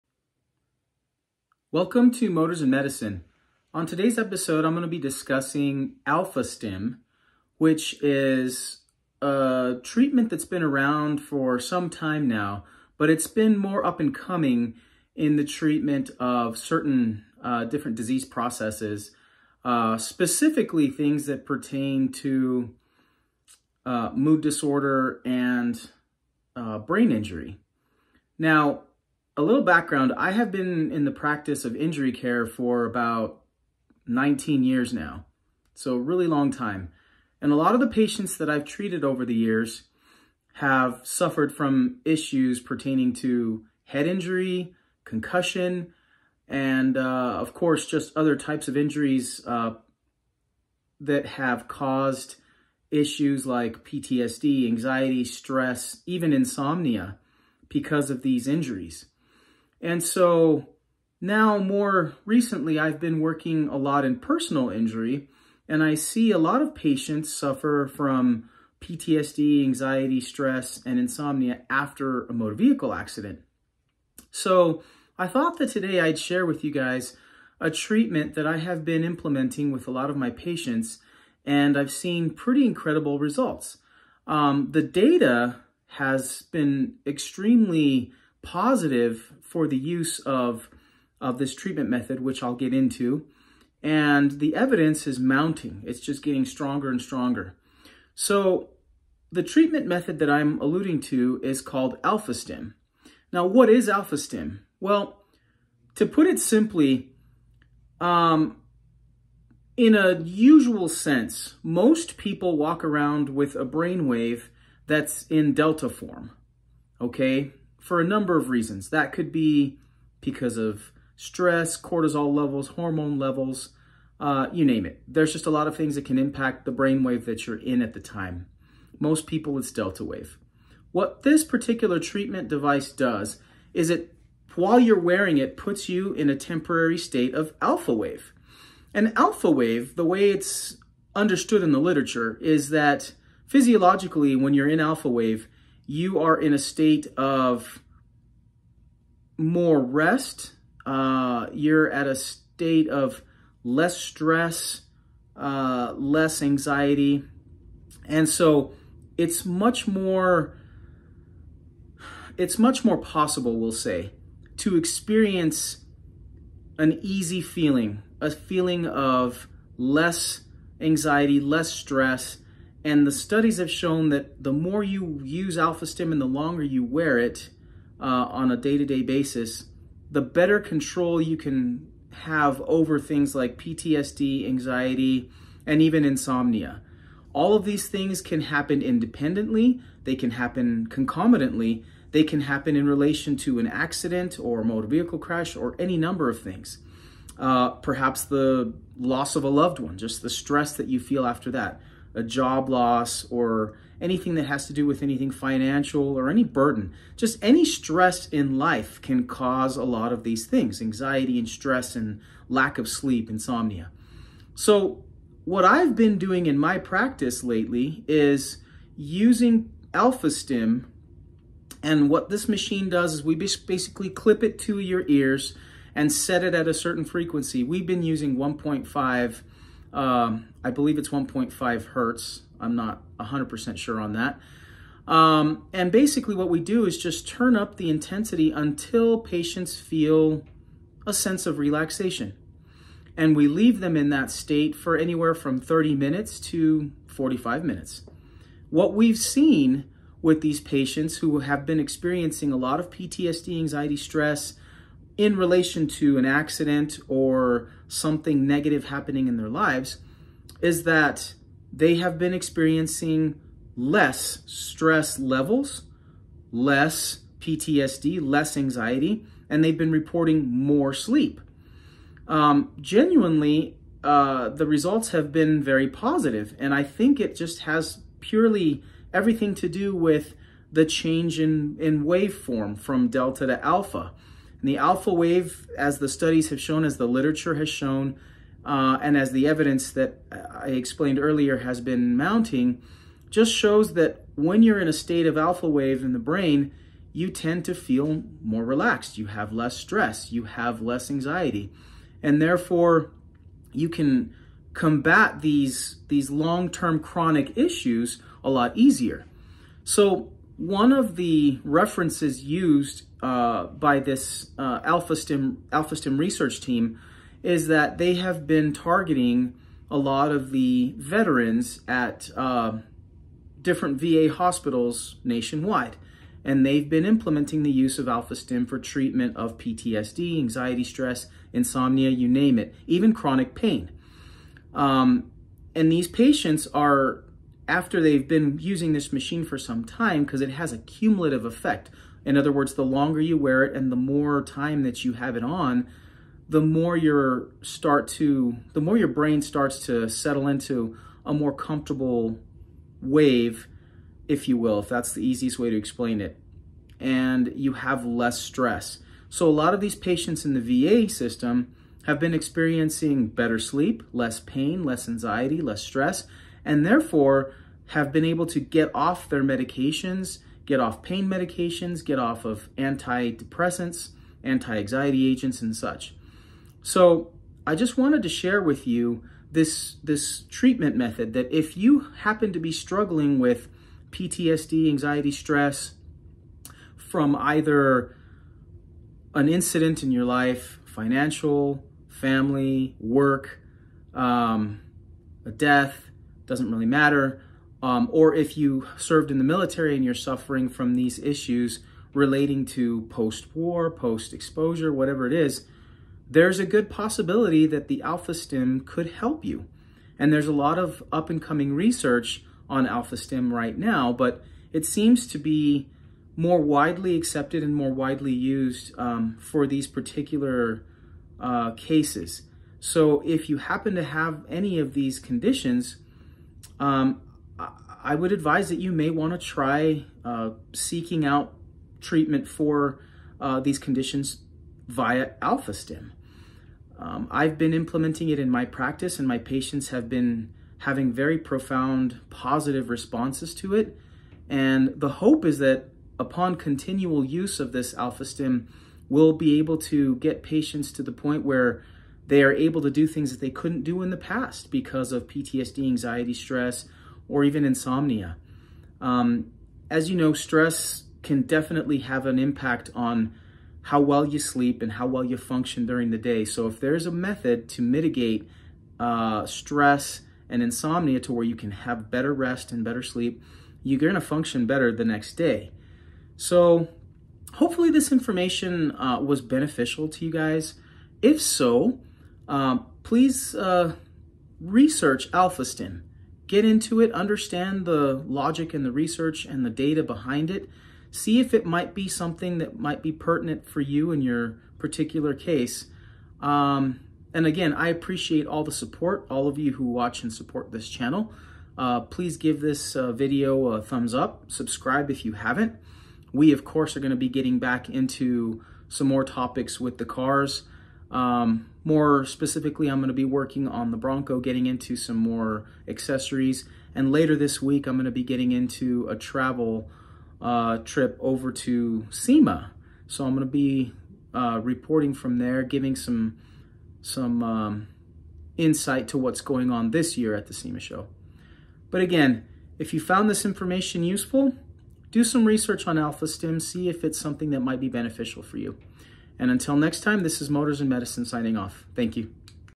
Alpha Stimulation can help treat PTSD, Anxiety and Insomnia!
On this video I discuss the use of Alpha Stim for treating PTSD and anxiety as well as insomnia.  It helps with brain injuries and helps reduce post concussion syndrome.